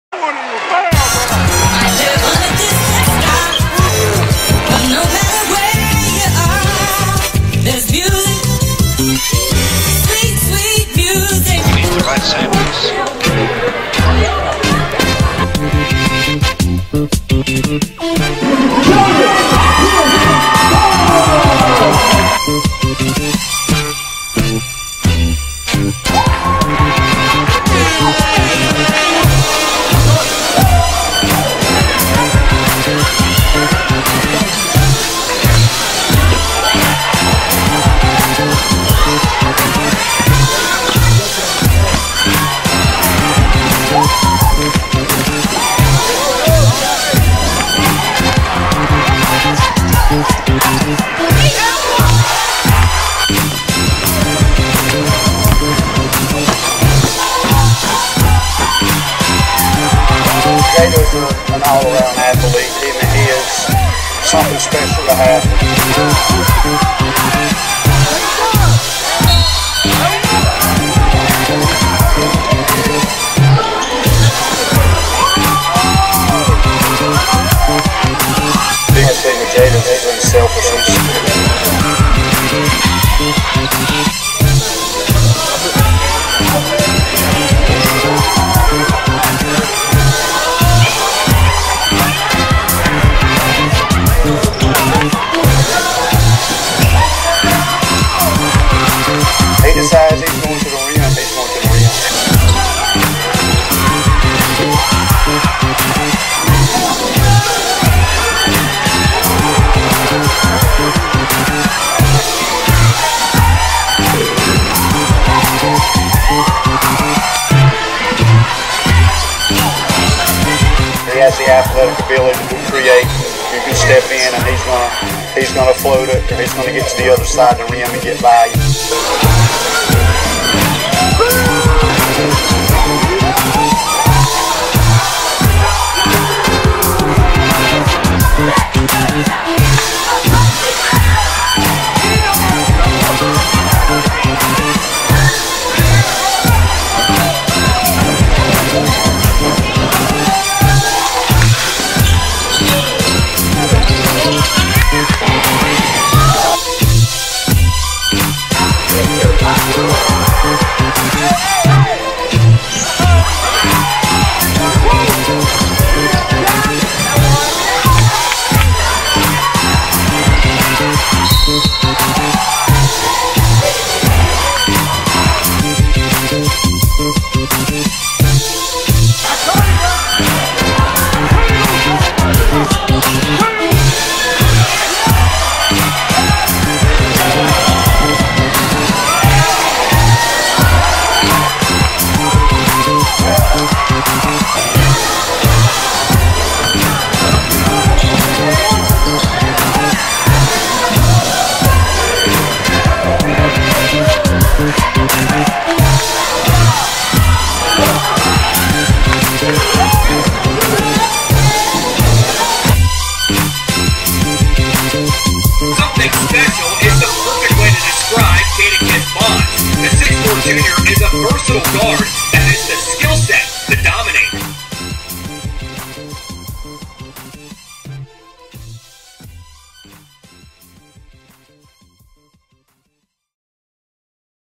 6'4 Jadakis Bonds | Riverside High (Williamston, NC) | Class of 2018
6'4 Jadakis Bonds | Riverside High (Williamston, NC) | Class of 2018

excellent ball handling, good court vision, very shifty, toughness, speed